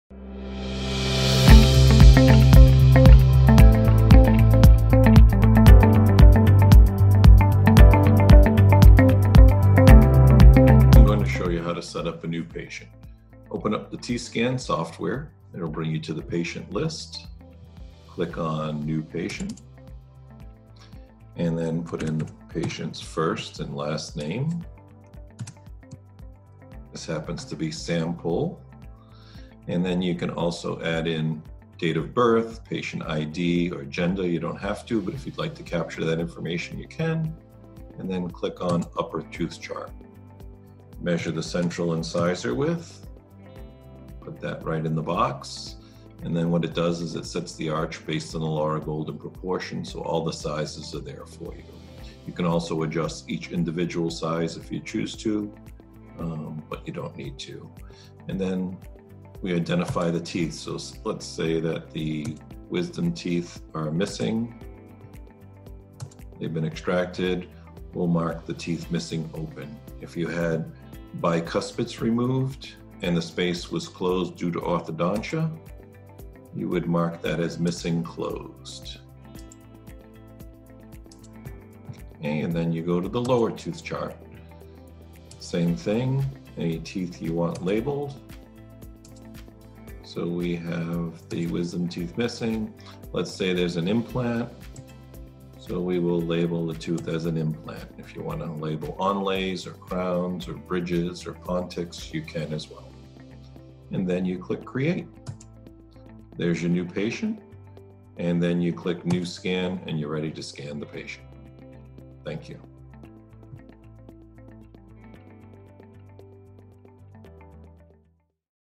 How to Create a New Patient Record in T-Scan Novus Software
In this video, Stephen Moniz, Regional T-Scan Sales Manager for Tekscan, covers step-by-step instructions for creating a patient recording, including:

- How to make adjustments on the upper and lower-tooth charts
- How to designate the patient's individual teeth for crowns, implants, removed teeth, and other statuses